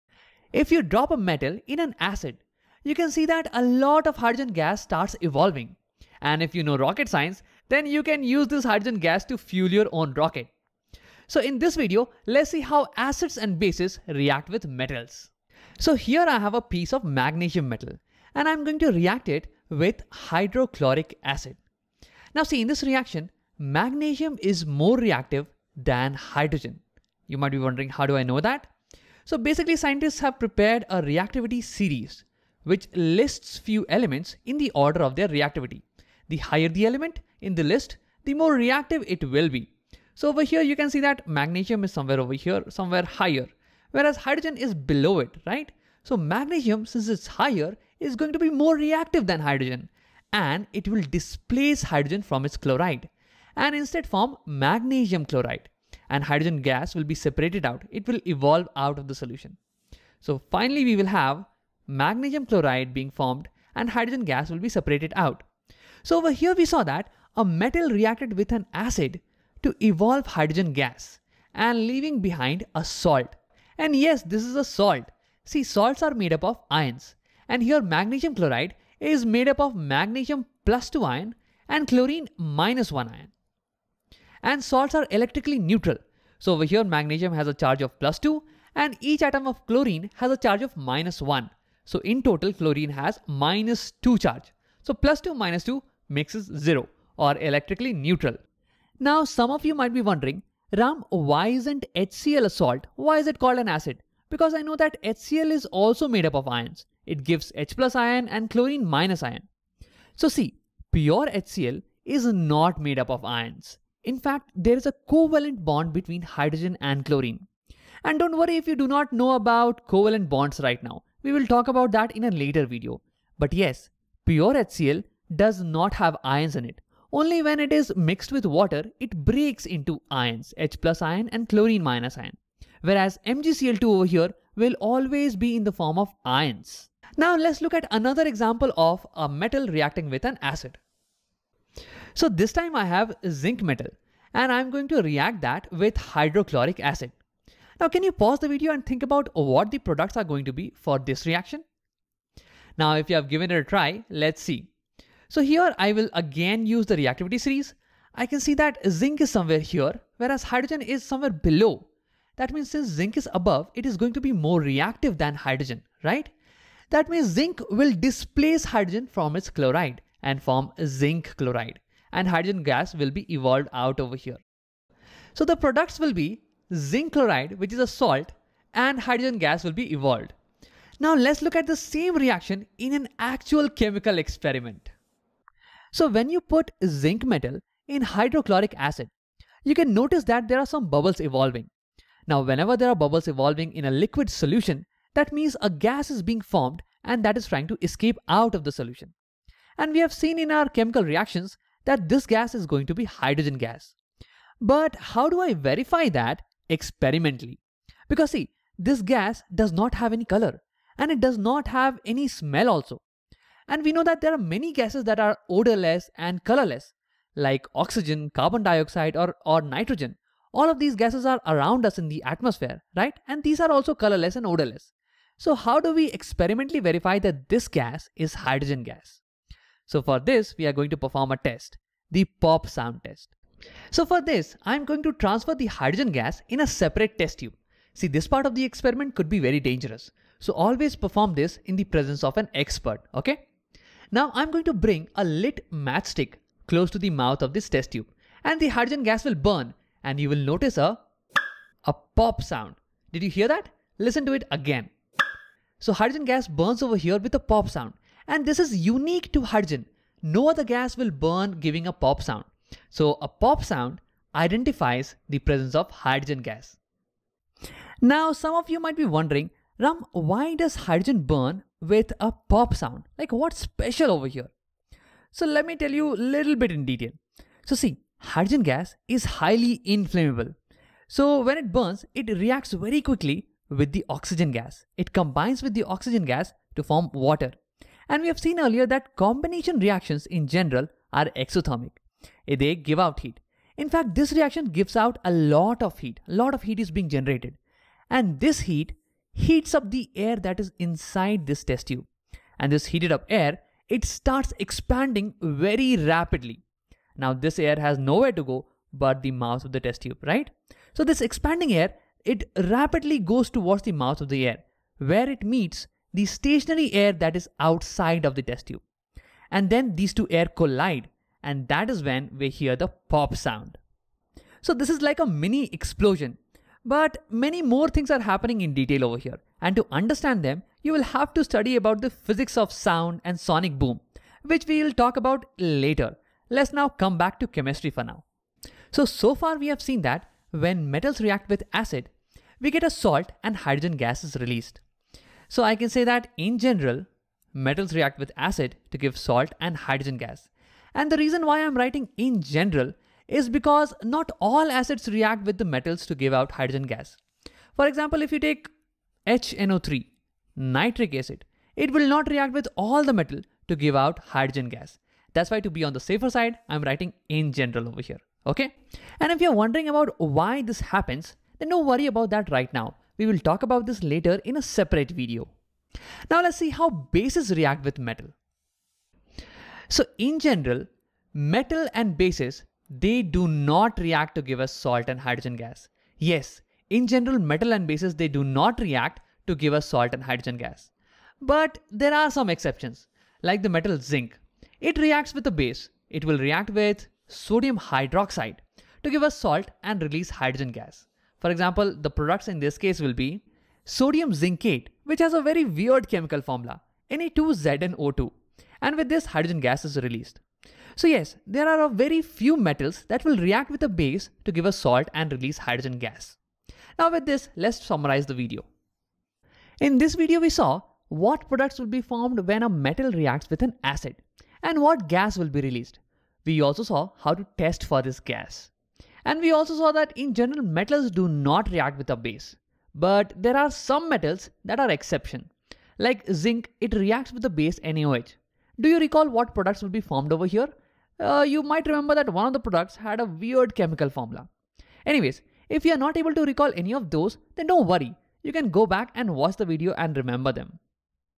Reaction of acids and bases with metals | Acids, bases, and salts | Chemistry | Khan Academy
In general, acids react with metals to give salt and release hydrogen gas. In general, bases do not react with metals and release hydrogen gas.